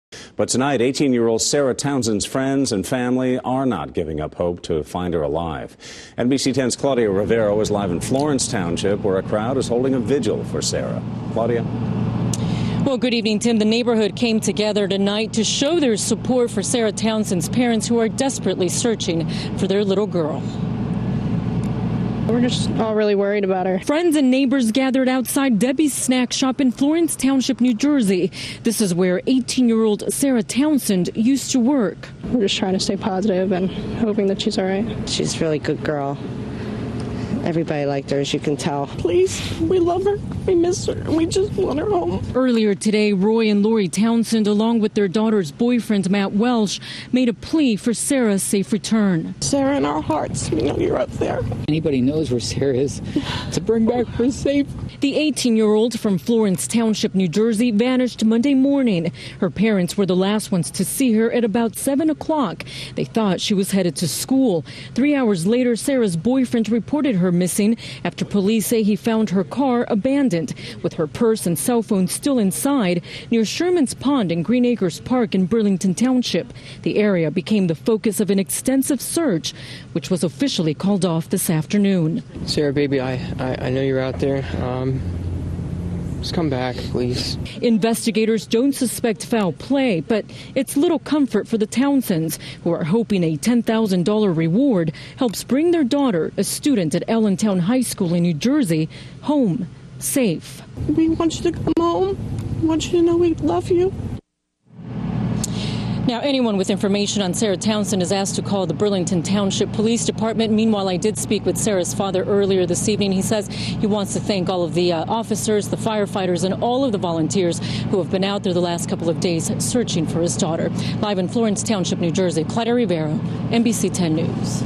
Family Begs for Sarah's Safe Return.
The search for missing 18-year-old Sarah Townsend was called off Tuesday but her loved ones were holding out hope and offering a reward for her safe return.